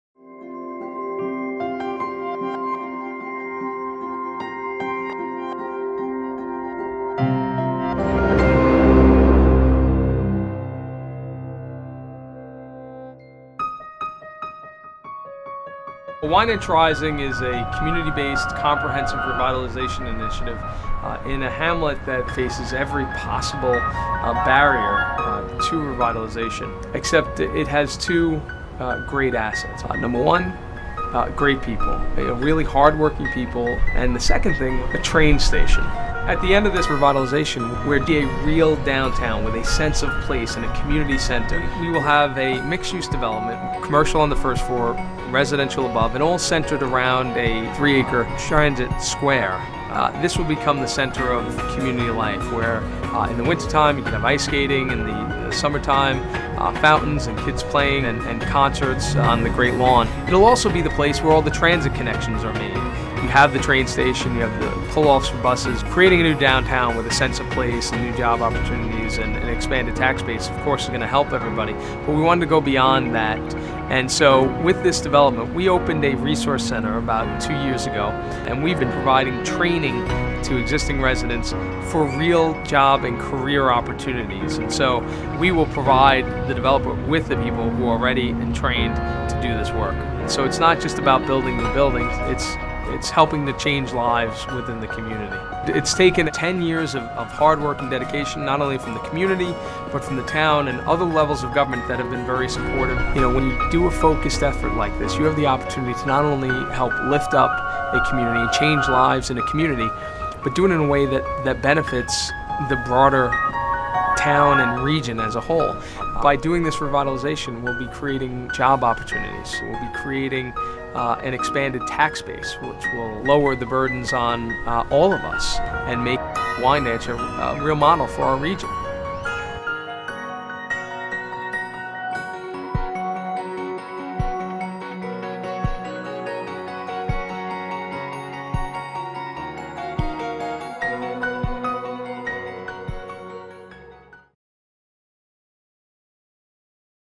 Supervisor Steve Bellone, Wyandanch Rising
Investments in long-term planning can promote economic growth! It's important we create good jobs so our kids can raise their kids here too.